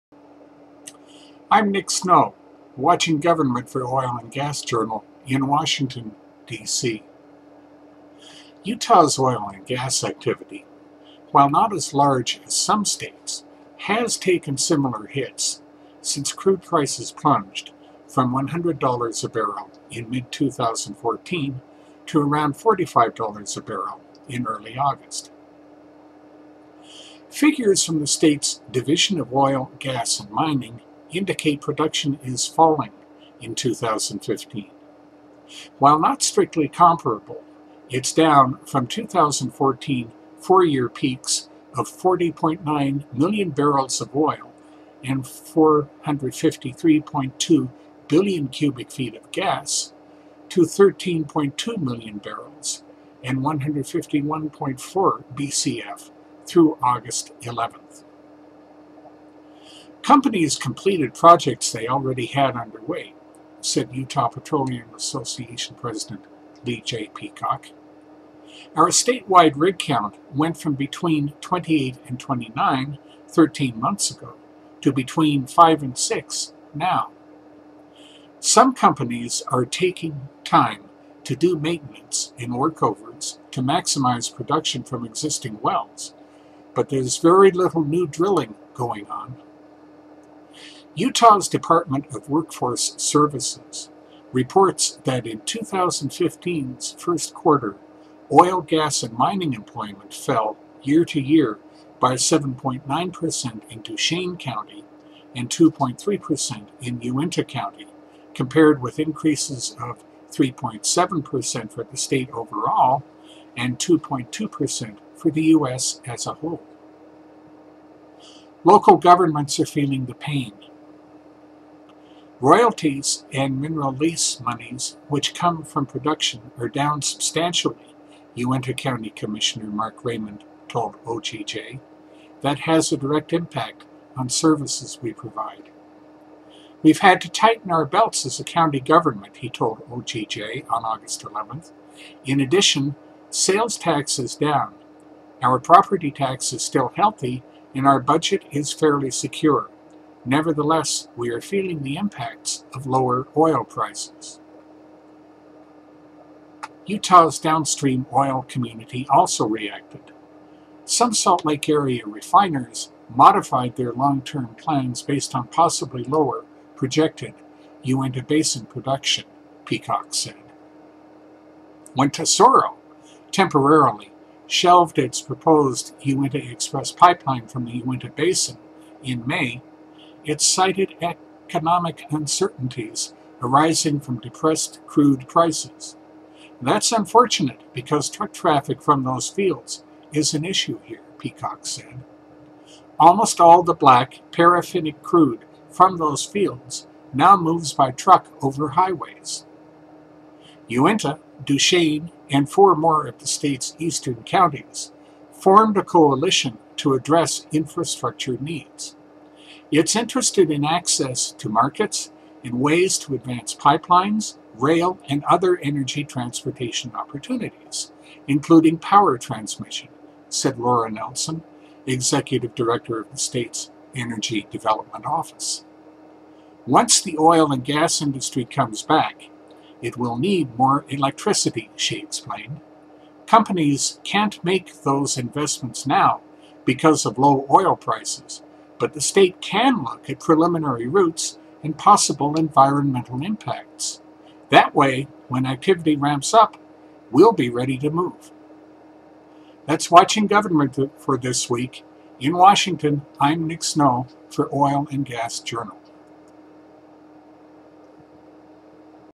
Utahns cope with low oil price
Utah’s oil and gas activity, while not as large as some states, has taken similar hits since crude prices plunged from $100/bbl in mid-2014 to around $45/bbl in early August. . . .